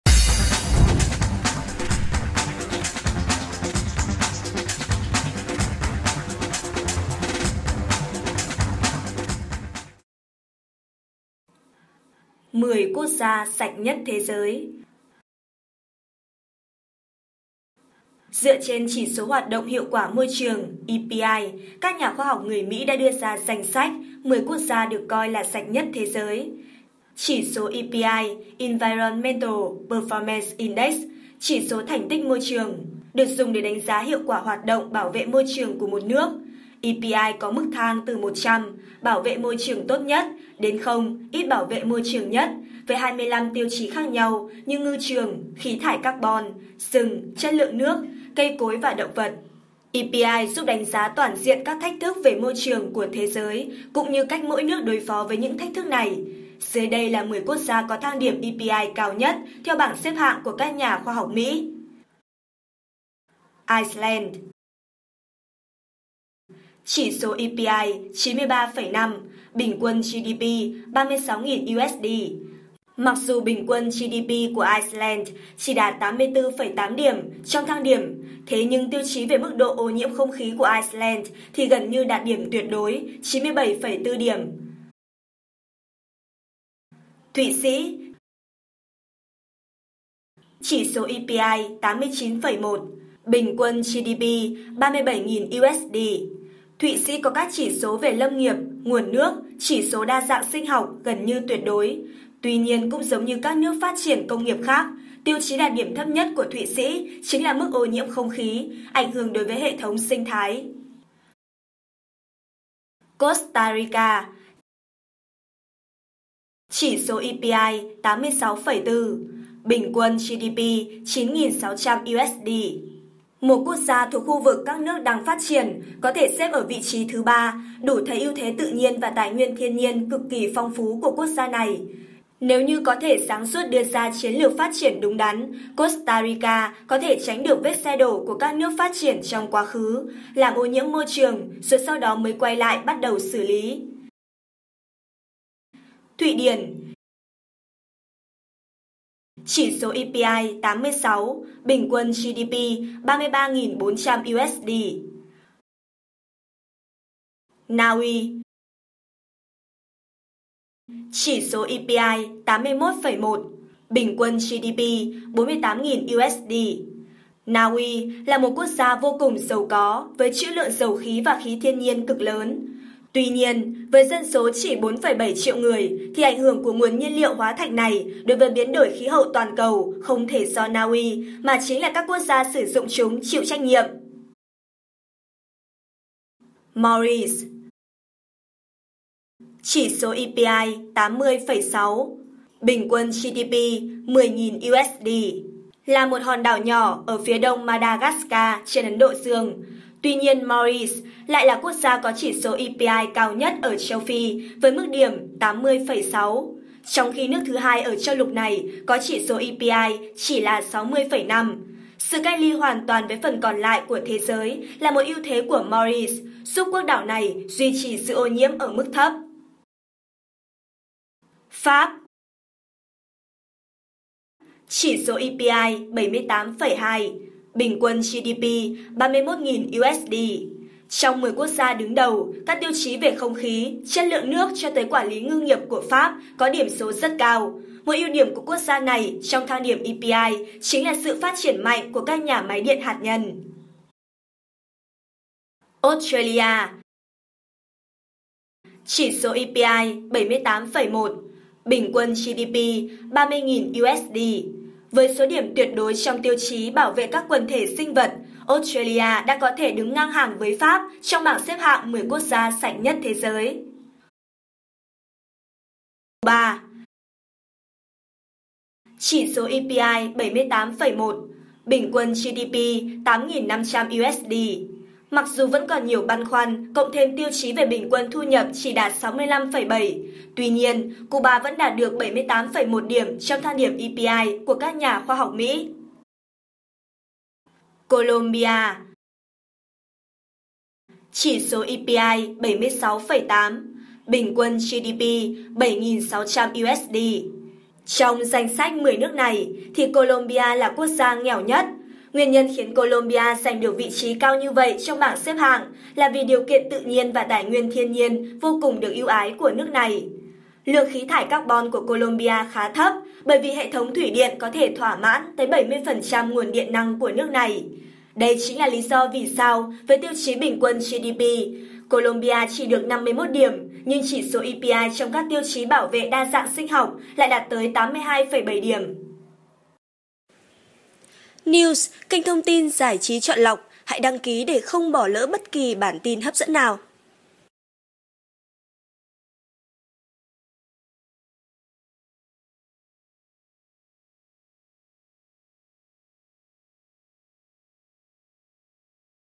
Top 10 quốc gia sạch nhất thế giới
[News TV] - Dựa trên chỉ số hoạt động hiệu quả môi trường (EPI), các nhà khoa học người Mỹ đã đưa ra danh sách 10 quốc gia được coi là sạch nhất thế giới.
EPI có mức thang từ 100 (bảo vệ môi trường tốt nhất) đến 0 (ít bảo vệ môi trường nhất) với 25 tiêu chí khác nhau như ngư trường, khí thải carbon, rừng, chất lượng nước, cây cối và động vật.
Dưới đây là 10 quốc gia có thang điểm EPI cao nhất theo bảng xếp hạng của các nhà khoa học Mỹ.
1. Iceland. Chỉ số EPI: 93.5 Bình quân GDP: 36.000 USD
2. Thụy Sĩ. Chỉ số EPI: 89,1 Bình quân GDP: 37.000 USD
3. Costa Rica. Chỉ số EPI: 86,4 Bình quân GDP: 9.600 USD
4. Thụy Điển. Chỉ số EPI: 86 Bình quân GDP: 33.400 USD
5. Na Uy. Chỉ số EPI: 81,1 Bình quân GDP: 48.000 USD
6. Maurice. Chỉ số EPI: 80,6 Bình quân GDP: 10.000 USD
7. Pháp. Chỉ số EPI: 78,2 Bình quân GDP: 31.000 USD
8. Australia. Chỉ số EPI: 78,1 Bình quân GDP: 35.000 USD
9. Cuba. Chỉ số EPI: 78,1 Bình quân GDP: 8500 USD
10. Colombia. Chỉ số EPI: 76,8 Bình quân GDP: 7.600 USD